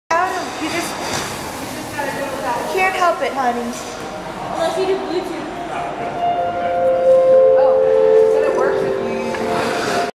hand sliding under a 9inch jig
So basically what you see is a pvc jig that is 4x 9inches.
If you know me you know what the dimensions go to & why i am doing this.
that being said , i look like such a dork :/